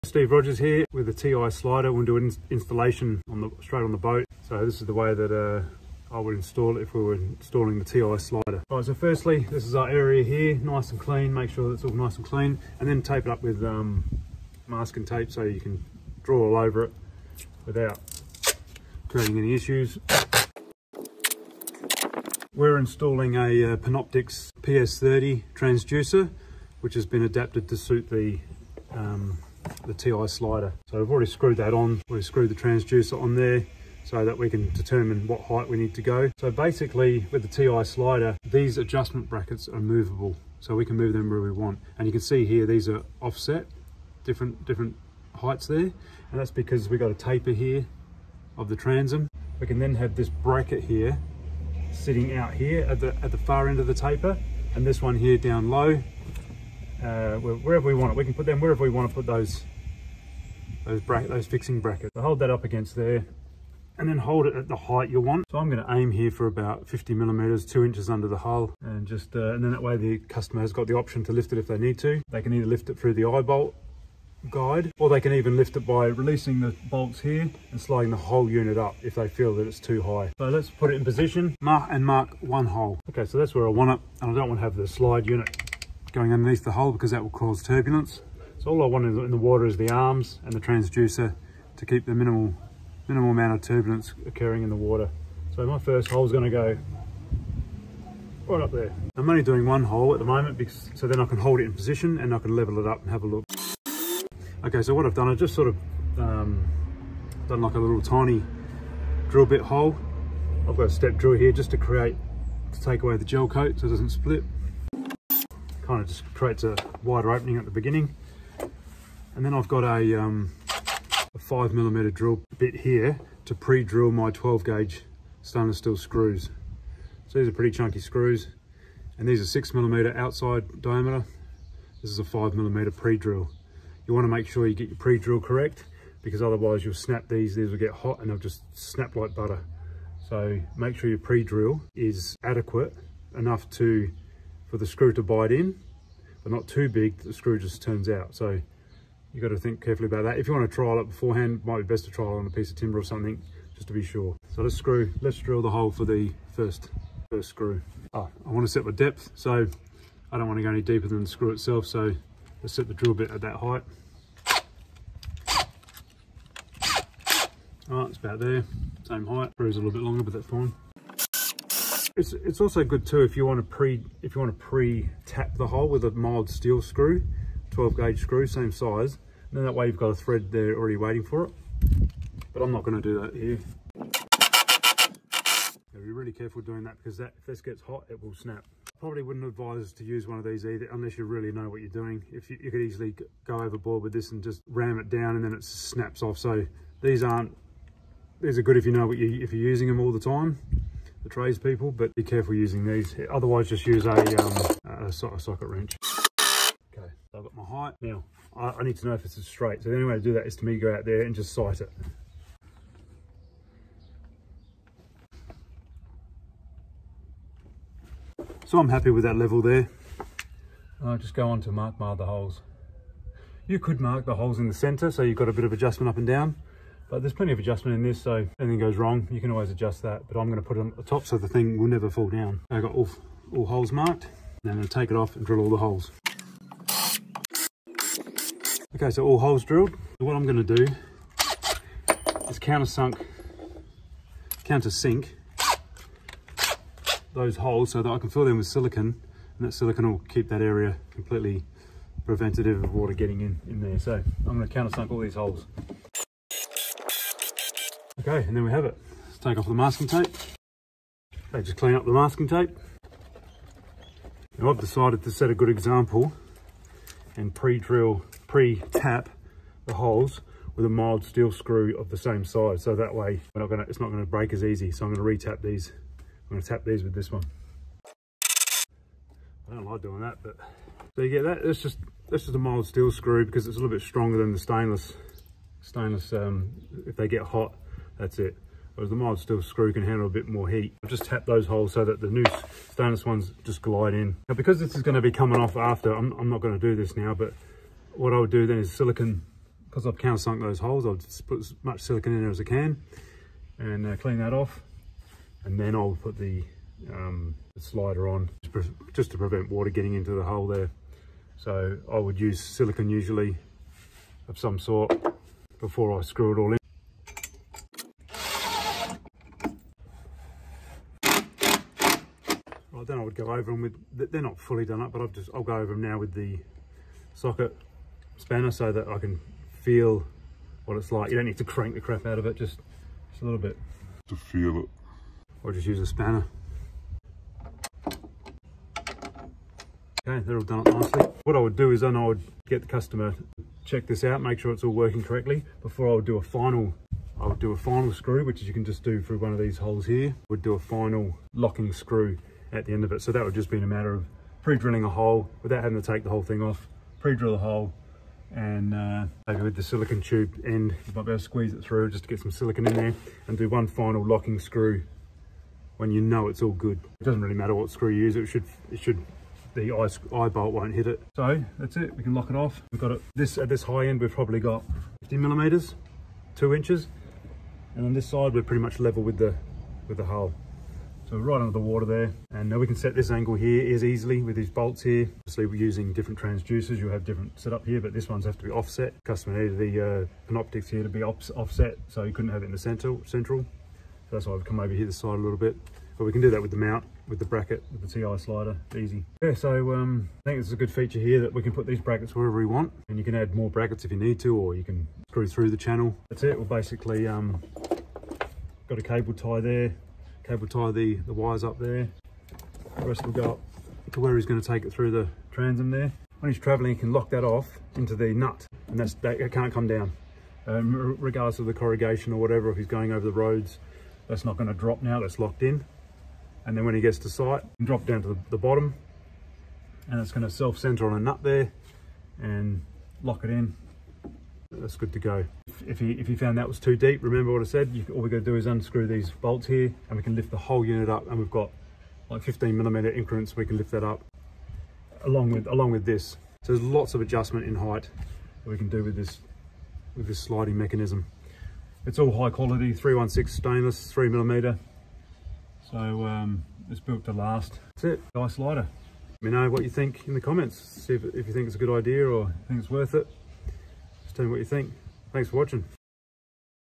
MARINE transducers- sliding bracket installation guide - this example using Garmin PS30 Panoptix
This is the ultimate guide to setting up your ti transom mounted transducer slide
Once you have established your best longitude line along your boat with least boat turbulence, the TI slide will get you any height required with plenty of adjustment options, including easy angle adjustment.
This example is using the Garmin Panoptix Ps30 transducer but the slide will suit other popular transducers such as the Furuno / Airmar 1 kw,
Garmin GT versions and Lowrance 3-1 transducers
Get in touch or check out the website at the end of the video
We ship anywhere
Thanks
Steve Rogers